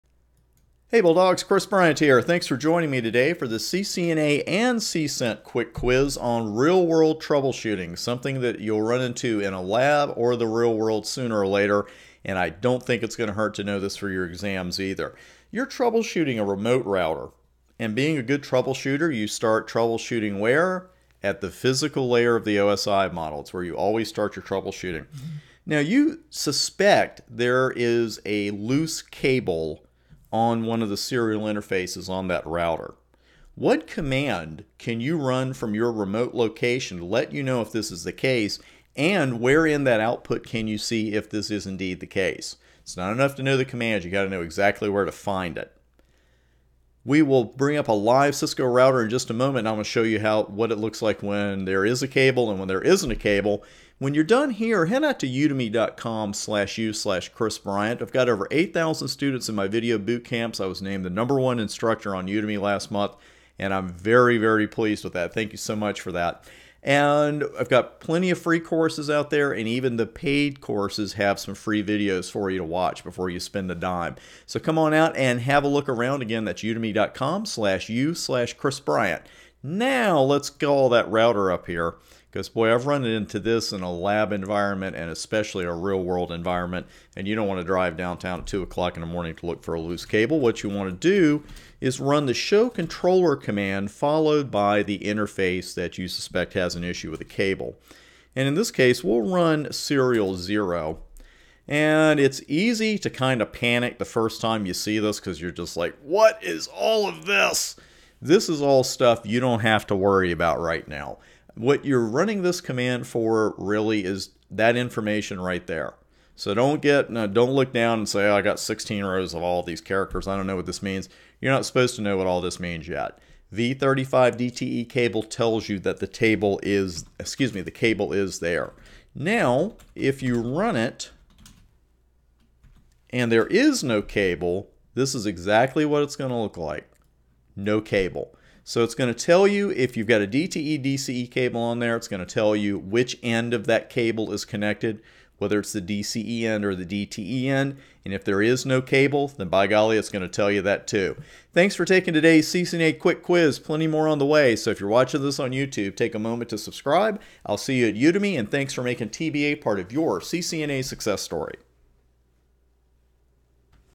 CCNA CCENT Quick Quiz: Real-World Troubleshooting
when you're done watching this video for a full list of my paid and free CCNA, CCNP, and CCNA Security courses!

Some free, some paid, all great!

In today's CCNA and CCENT Quick Quiz, I'll present you with a real-world troubleshooting scenario, and then show you just how to solve it -- all demoed on live Cisco routers, as always!

Chris Bryant
CCIE #12933
"The Computer Certification Bulldog"